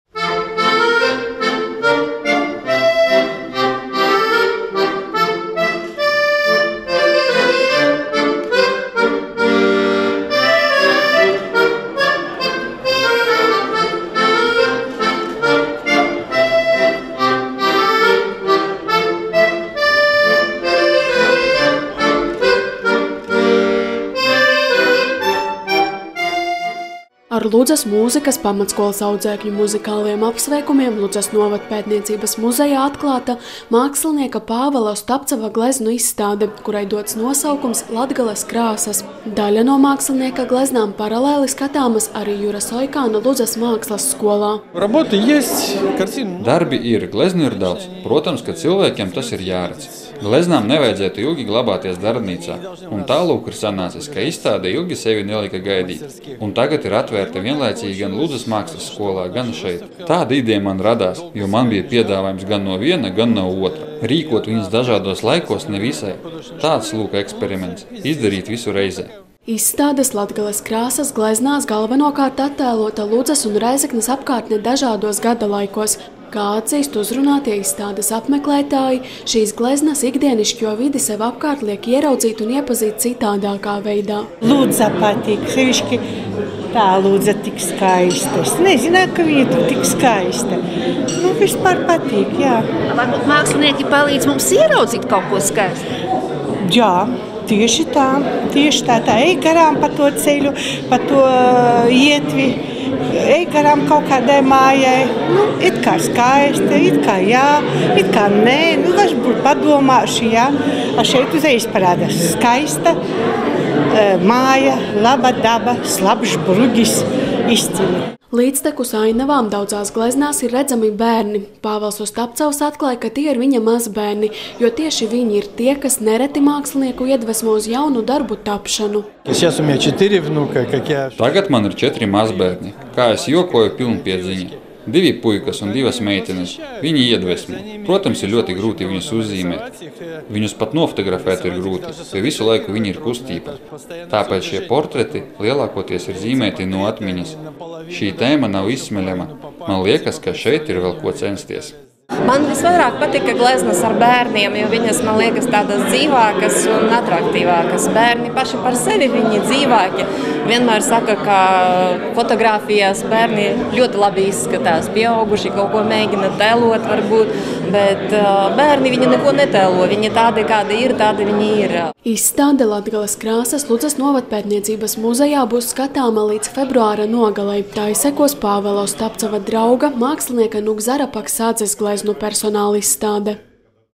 Ludzā atklāta Pāvela Ostapceva gleznu izstāde
Mākslinieks Pāvels Ostapcevs dzimis Rēzeknē, bet savās gleznās ataino ainavas no dažādām Latgales vietām. Šobrīd daļa no mākslinieka mīļākajām Latgales ainavu gleznām aplūkojamas Ludzas Novadpētniecības muzejā un Jura Soikāna Ludzas mākslas skolā.